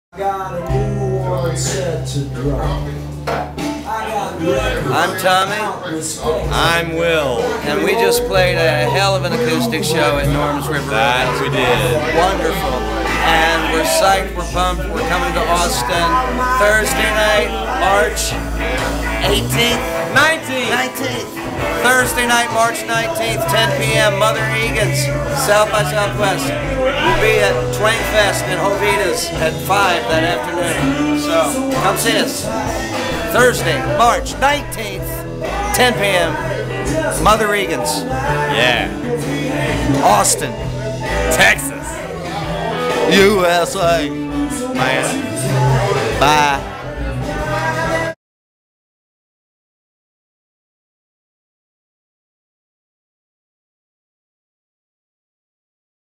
DADDY (Will Kimbrough & Tommy Womack) pyshced for SXSW 2009!
DADDY (Will Kimbrough & Tommy Womack) Talking about their upcoming shows in Austin, TX; officially playing SXSW in March 2009!